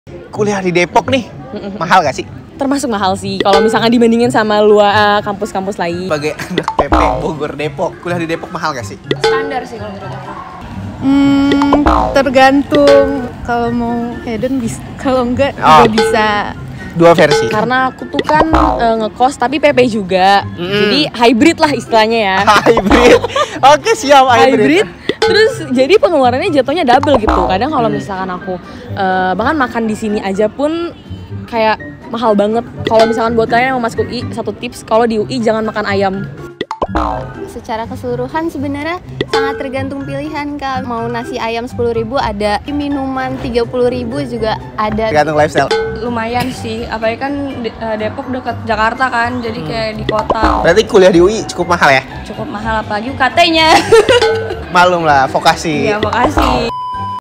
KULIAH DI UI MAHAL GAK SI? TERGANTUNG GAYA HIDUP TERNYATA!!
BANYAK ANAK-ANAK SMA SEDERAJAT YANG MAU MASUK UI, TAPI MEREKA PENASARAN KULIAH DI UI MAHAL ATAU GAK

TERNYATA JAWABANNYA TERGANTUNG "LIFESTYLE KAMU SELAMA KULIAH" KALO BUAT UANG KULIAH SI MURAH-MURAH AJA KARNA ADA SISTEM BOP BERKEADILAN, TAPI KALO PROGRAM VOKASI, PARAREL SAMA INTERNASIONAL YA PASTI AGAK LEBIH MAHAL